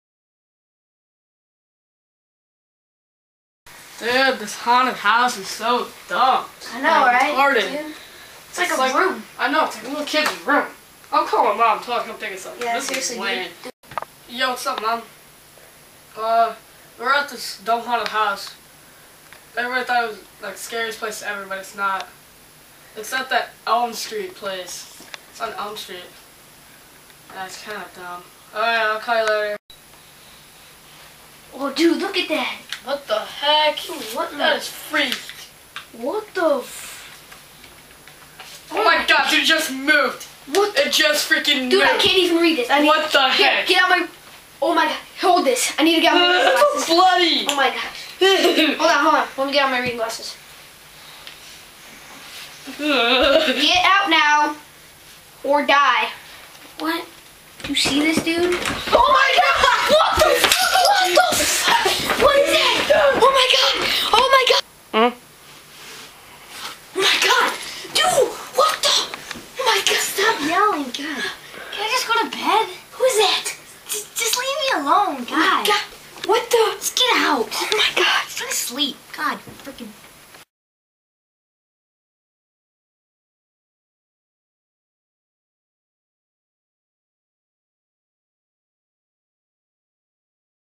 Haunted House
jake and riley are in a haunted bedroom